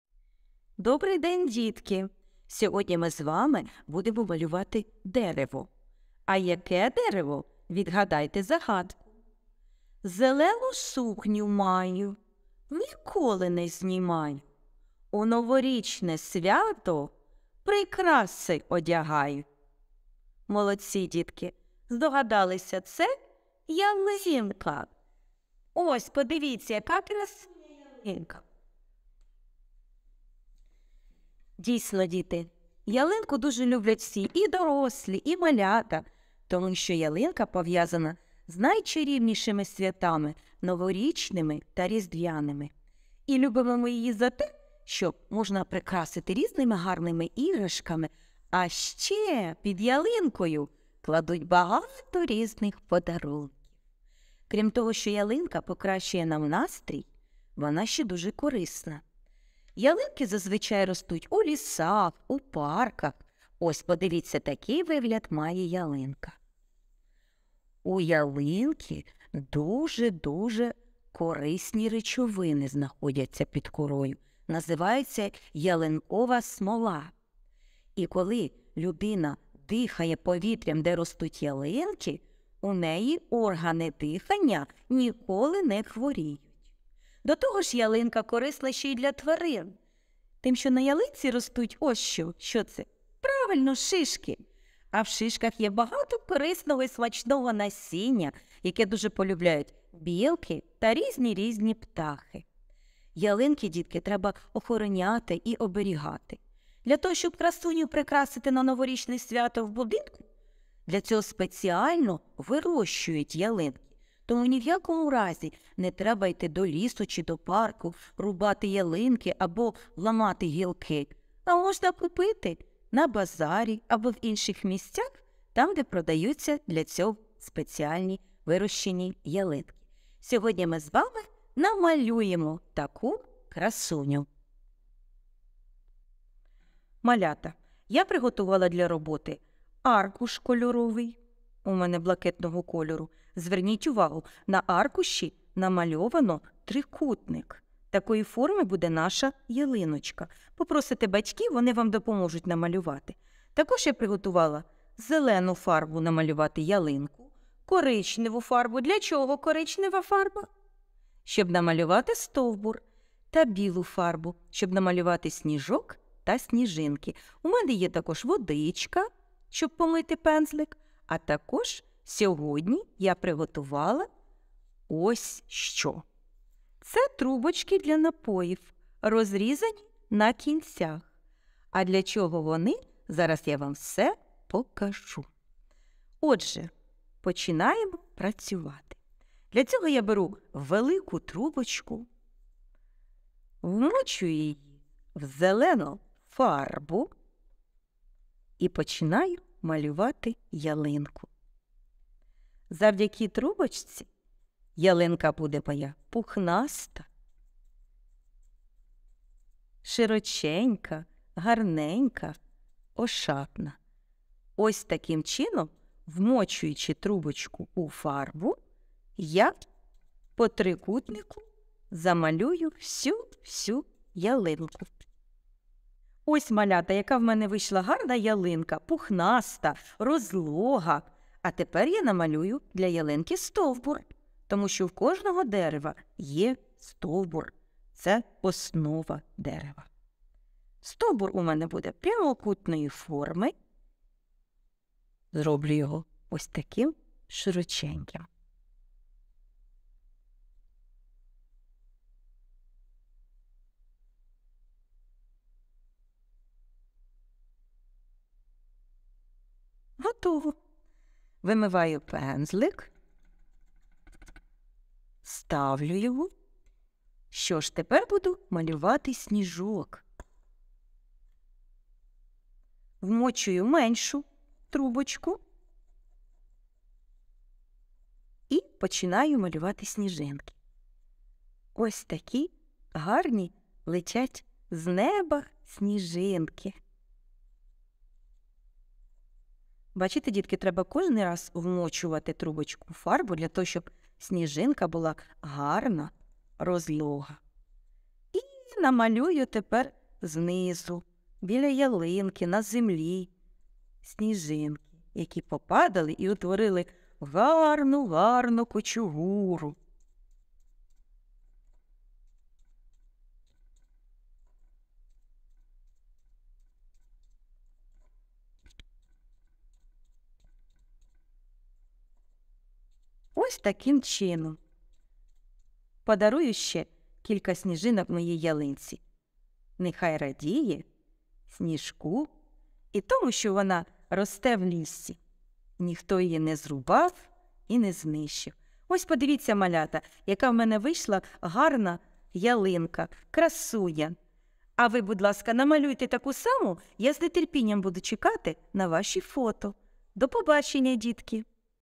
Нетрадиційне малювання "Новорічна Ялинка"
Сьогодні ми з вами малюватимемо дерево, а яке саме дерево - відгадайте загадку: "зелену сукню маю, ніколи не знімаю, у новорічне свято прикраси надягаю". Звичайно ж, це Ялинка - дерево, яке ми всі так любимо за його пов'язаність з найчарівнішими святами, такими як Різдво та Новий Рік. Ми прикрашаємо ялинку різними гарними іграшками та кладемо під неї багато подарунків. Проте, ялинка не тільки прикрашає наше життя, а й корисна для нашого здоров'я та довкілля.

Зазвичай ялинки ростуть у лісах та парках, і містять корисні речовини, такі як ялинкова смола, що знаходиться під корою. Ця смола відома своїми протизапальними та антисептичними властивостями, тому вона широко використовується в народній медицині та фармації. Коли людина дихає повітрям, де ростуть ялинки, її органи дихання ніколи не хворіють.

Ялинки є також корисними для тварин. На ялинці ростуть шишки з багато корисного та смачного насіння, яке полюбляють білки та різні птахи. Це є однією з причин, чому ялинки є важливим елементом екосистеми лісу.

Щоб мати змогу прикрасити ялинку на Новорічне свято, спеціально вирощують ялинки, тому не потрібно йти до лісу або парку та рубати ялинки або ламати гілки. Ялинки можна купити на базарі чи в інших місцях, де їх продають. Проте, важливо охороняти та оберігати ялинки, щоб вони продовжували прикрашати наше життя та були корисними для природи. Наприклад, якщо ви вирішили придбати ялинку, переконайтеся, що вона була вирощена екологічно та безпечно